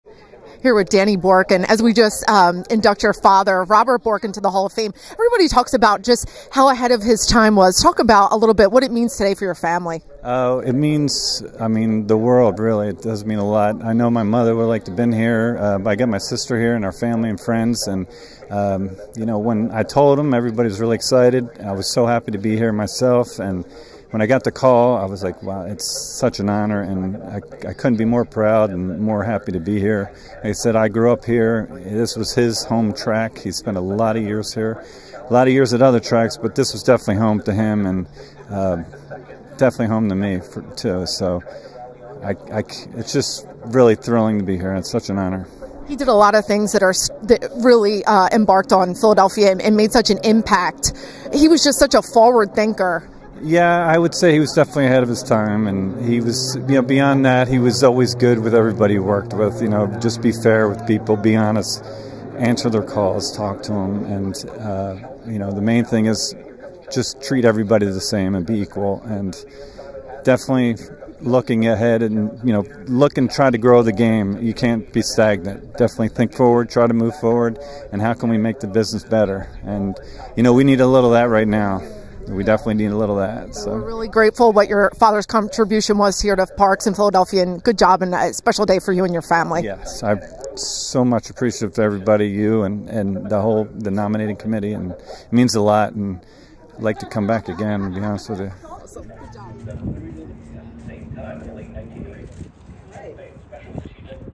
Robert Bork - Parx Racing Hall of Fame induction
We speak with Robert Bork’s son Daniel Bork on being inducted into our the Parx Racing Hall of Fame for Special Achievement on 10/18/22